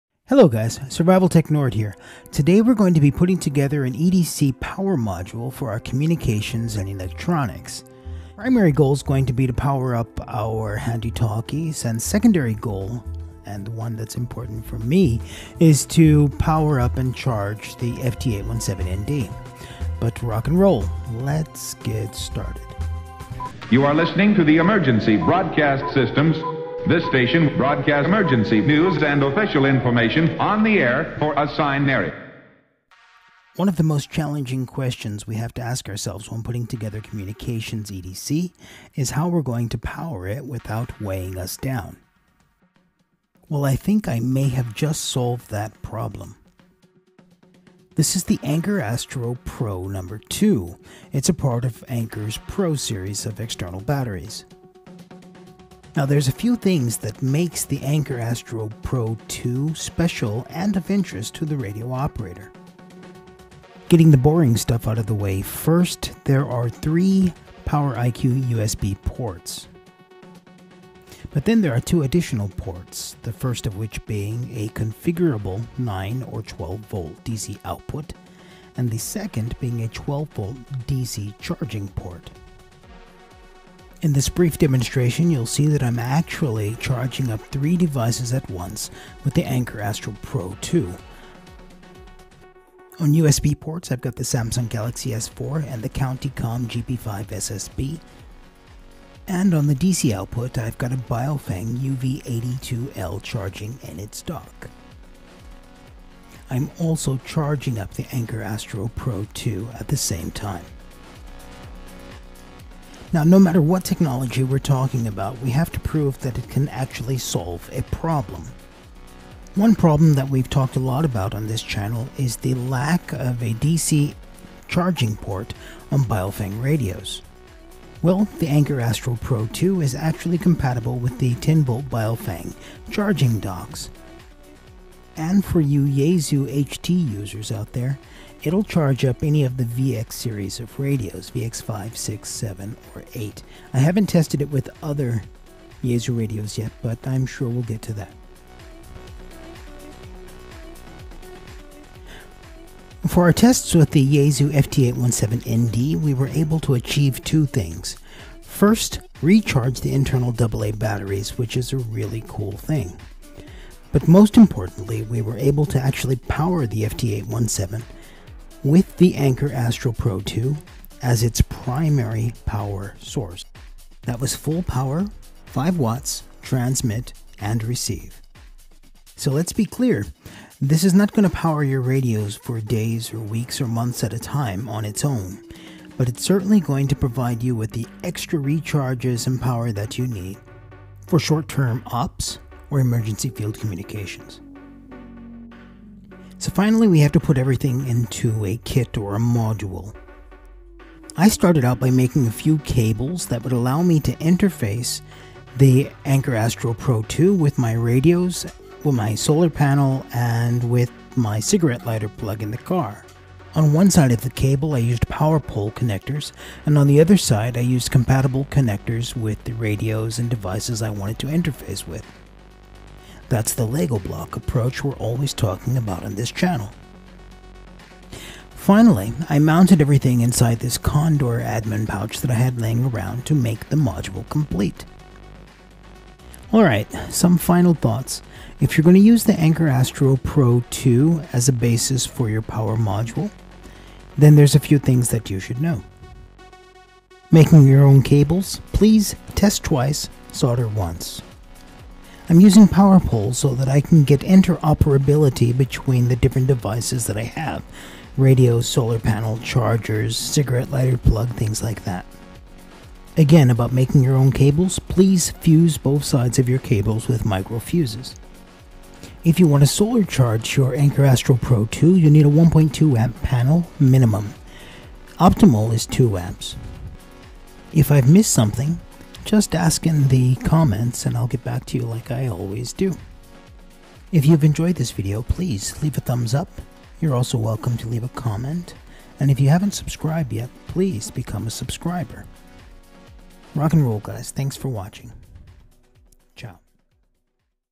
QRP Battery Pack Anker Pro2 for Ham Radio
This EDC power Module came about from the need to have a lght weight, external battery pack which could charge or maintain our HT radios, and FT-817ND.
The core of the module is the Anker Astro Pro2 20A external battery pack. It has three Power IQ USB ports, one 9/12v DC output, and one DC12v charging port. One of its key functions is its ability to power devices, and itself be charged simultaneously.

In the video we show testing done with the Baofeng UV-82L, Yaesu VX-5R, Yaesu FT-817ND, and various USB powered devices. Performance was as expected, and no problems were encountered during testing of any of the comms gear.

Food for thought. Amazon has a second version of the Anker Astro Pro2 now. It has a single USB port, and two DC outputs, but capacity and functionality are the same.

The Anker Astro Pro2 can be charged with a cigarette lighter plug from a wallwart, from a solar panel,  power supply, order any number of DC power sources.
Planning on purchasing from Amazon?

Ultimately, I decided to make my own cables for my various radios and electronic devices. I used barrel connectors and powerpoles to continue my Lego block approach to system interoperability. this is working well, as dissimilar products from various manufacturers, can be used as if they were made by a single entity.

I'll report on the long term robustness of the Anker Astro Pro2 as time goes on.

sadly the anchor to is no longer available, and no cost-effective solution has been found. so I decided to start my own project

This means that, at zero cost to you, I will earn an commission if you click through the link and finalize a purchase.